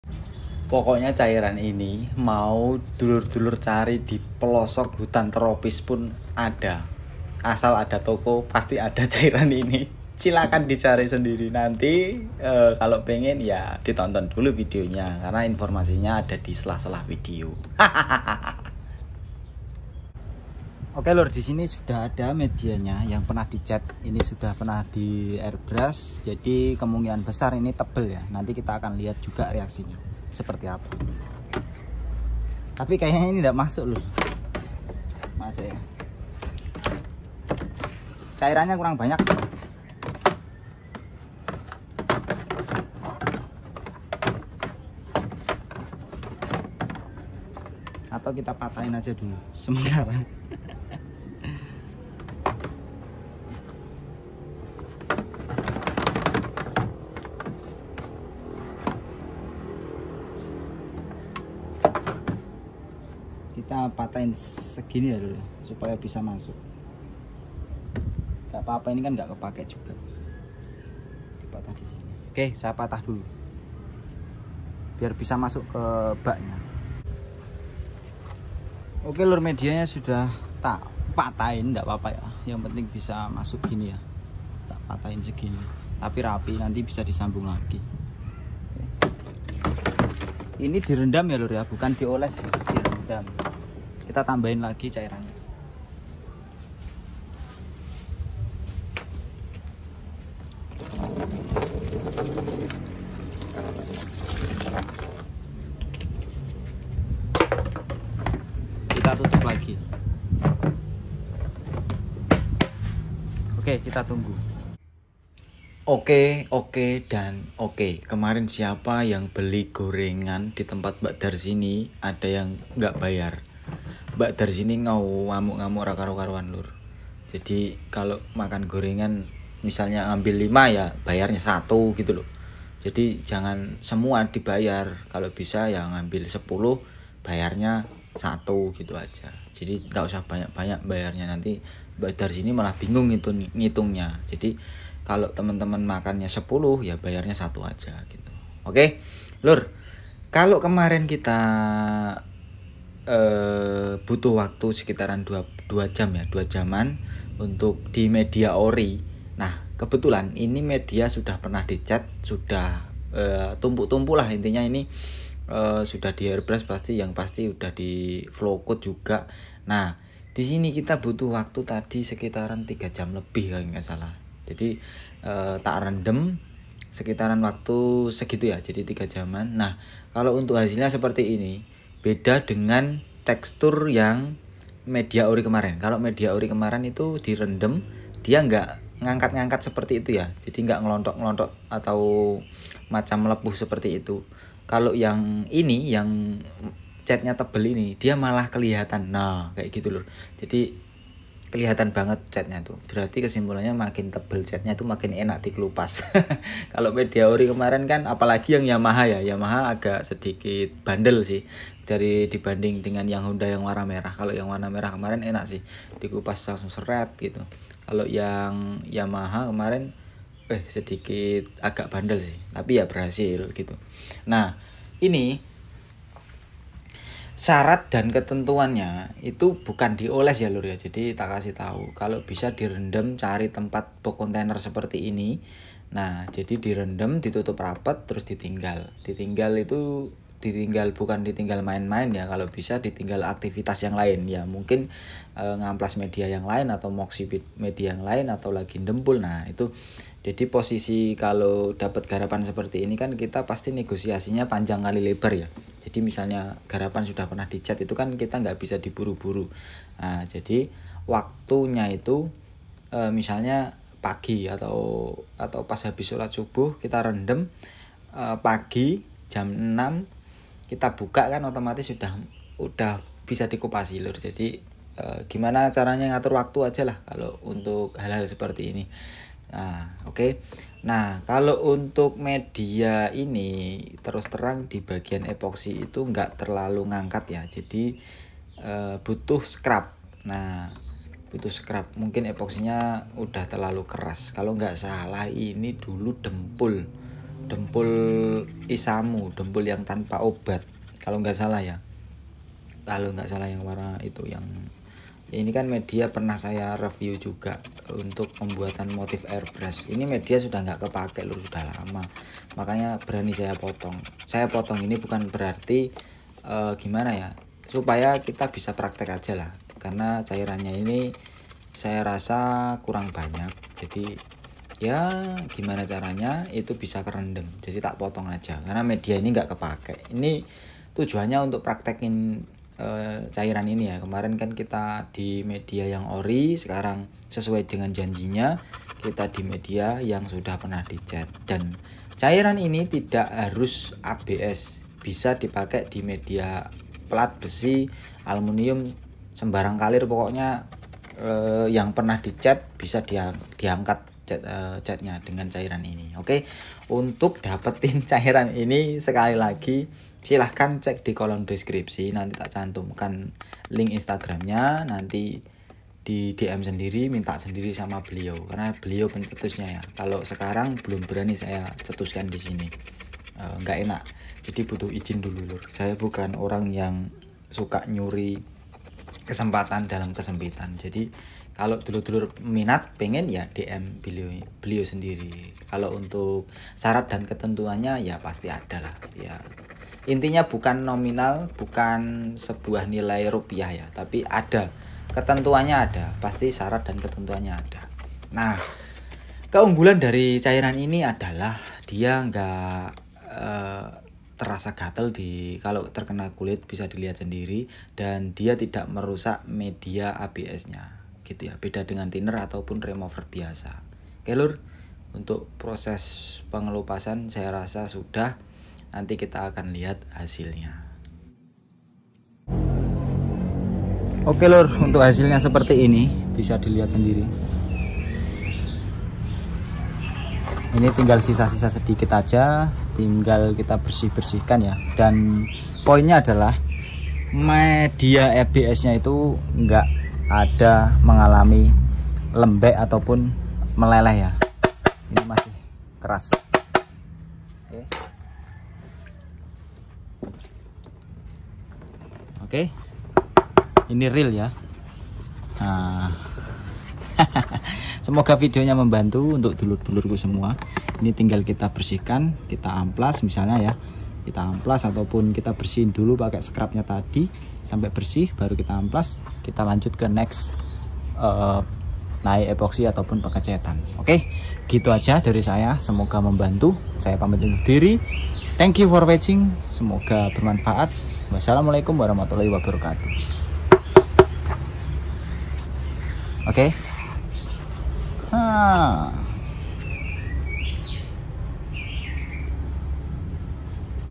KUPAS CAT BERLAPIS DI MEDIA PLASTIK ATAU ABS AMAN TIDAK RUSAK ! RAMUAN DARSINI
Oke lur kita lanjut tes yang ke dua di media yang sdah pernah di cat seperti yang dulur2 lihat
Cairan ini sangat berfungsi dengan baik
 Di samping murah mudah di dapat cairan ini tidak merusak media plastik dan jga bisa di gunakan di media metal almunium bahan lainya yang sdah pernah di cat

Seperti biasa informasinya klo dulur2 mau
 Tau cairan ini
 Silahkan dulur2 DM ke insatagram yang saya cantumkan link berikut ini

Terimakasih atas perhatianya